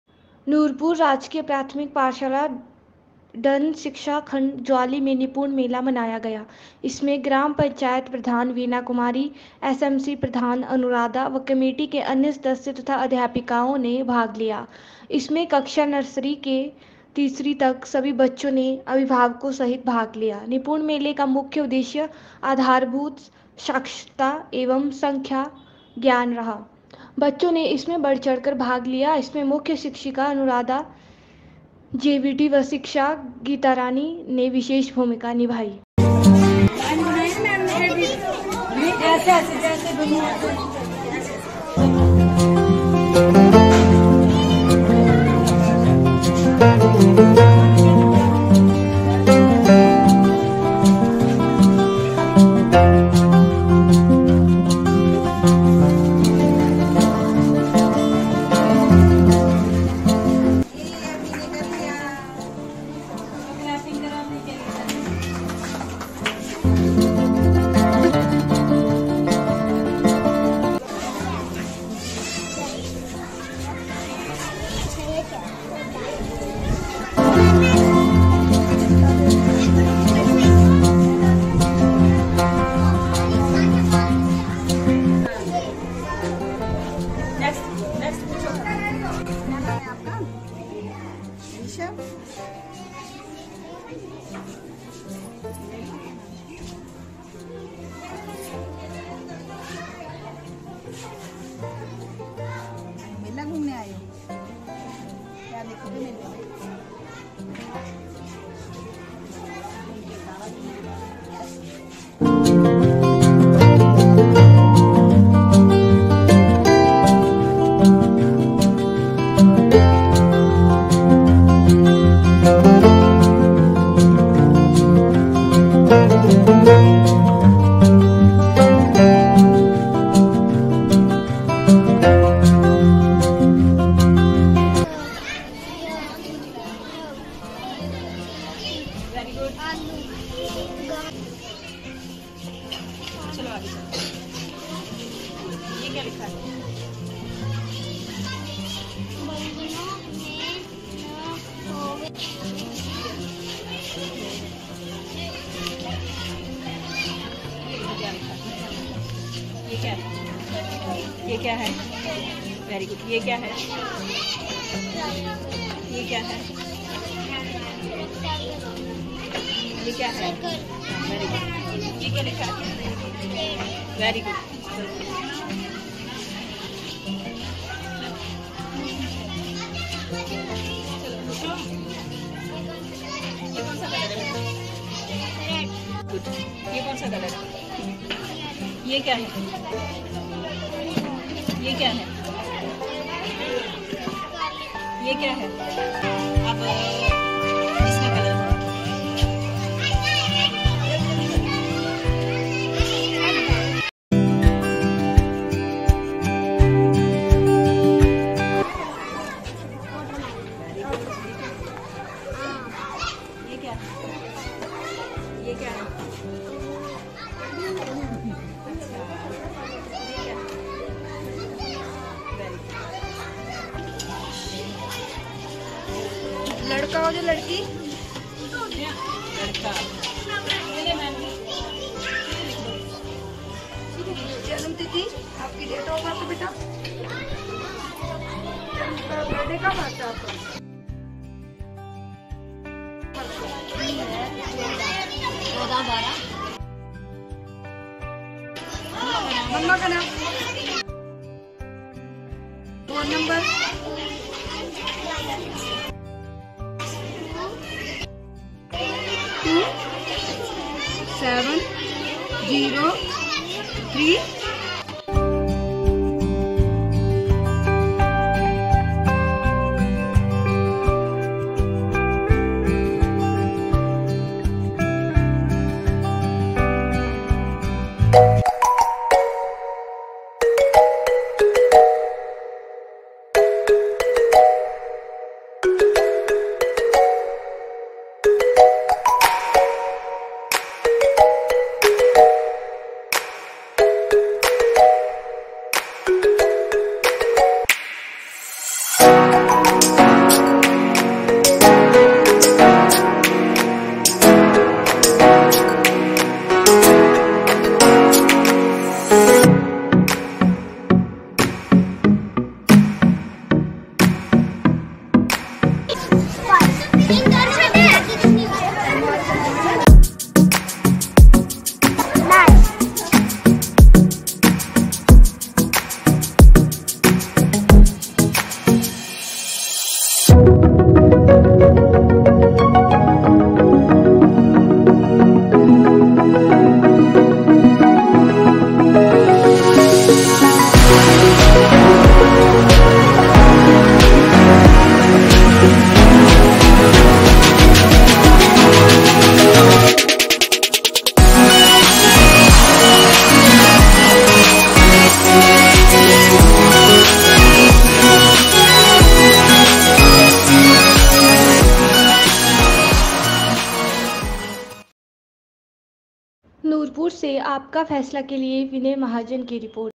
नूरपुर राजकीय प्राथमिक पाठशाला ढन शिक्षा खंड ज्वाली में निपुण मेला मनाया गया
इसमें ग्राम पंचायत प्रधान श्रीमती वीणा कुमारी, एस एम सी प्रधान श्रीमती अनुराधा व कमेटी के अन्य सदस्य तथा अध्यापिकाओं ने भाग लिया। इसमें कक्षा नर्सरी से तीसरी तक सभी बच्चों ने अभिभावकों सहित भाग लिया । निपुण मेले का मुख्य उद्देश्य आधार भूत साक्षरता एव संख्या ज्ञान रहा। बच्चों ने इसमें बढ़ चढ़ कर भाग लिया। इसमें  मुख्य  शिक्षिका अनुराधा जेवीटी व शिक्षिका गीतांरानी  ने विशेष  भूमिका निभाई।